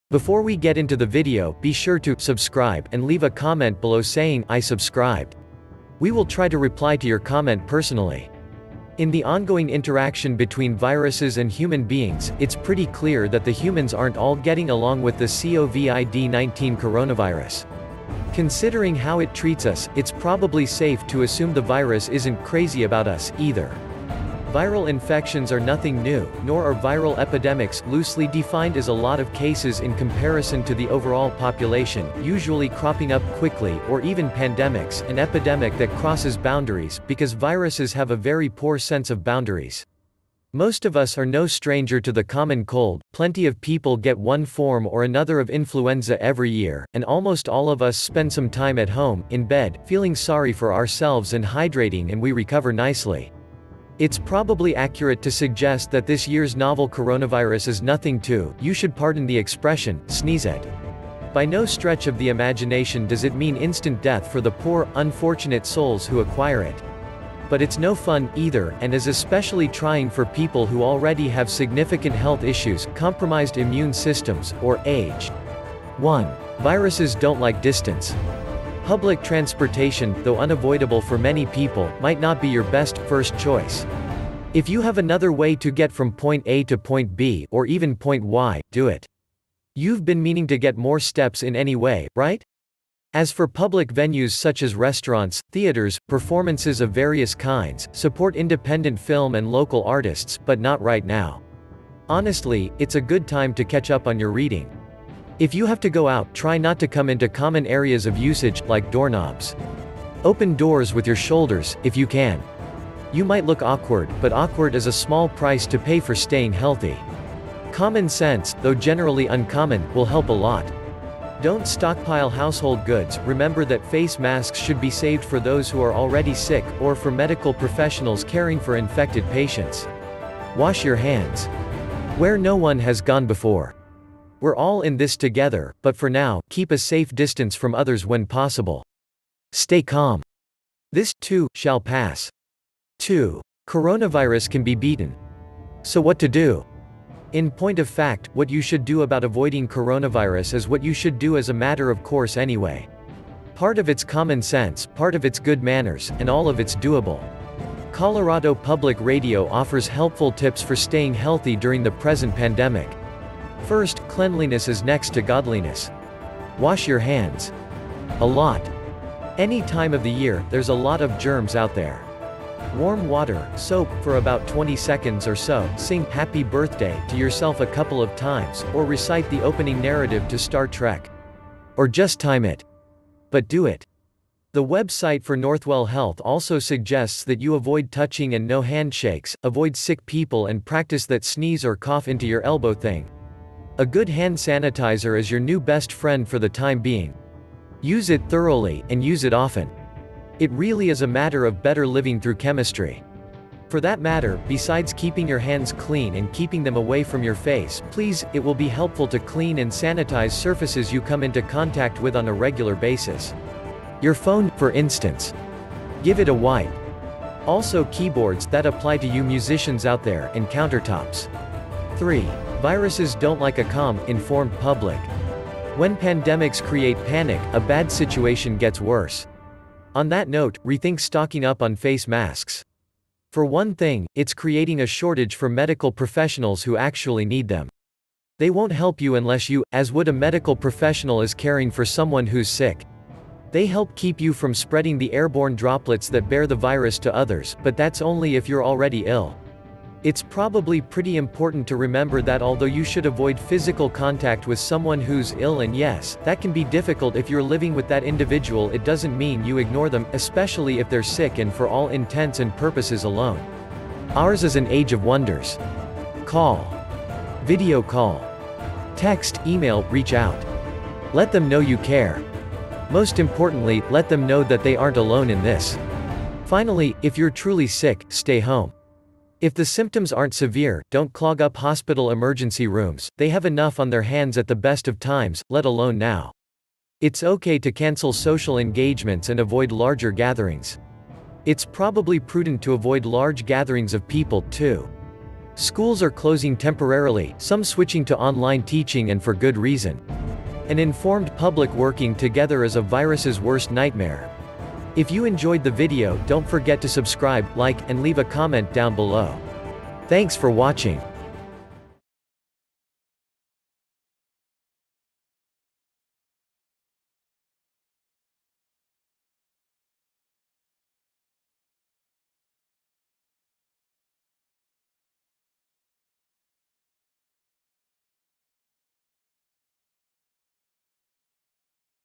3 Things Coronavirus Supposedly Does Not LIKE!
This video explains the 3 things coronavirus supposedly does not like. In the ongoing interaction between viruses and human beings, it's pretty clear that the humans aren't all gaga about the new kid on the block, the COVID-19 coronavirus. Considering how it treats us, it's probably safe to assume the virus isn't crazy about us, either. Viral infections are nothing new, nor are viral epidemics (loosely defined as a lot of cases in comparison to the overall population, usually cropping up quickly) or even pandemics (an epidemic that crosses boundaries, because viruses have a very poor sense of boundaries). Most of us are no stranger to the common cold; plenty of people get one form or another of influenza every year; and almost all of us spend some time at home, in bed, feeling sorry for ourselves and hydrating and we recover nicely.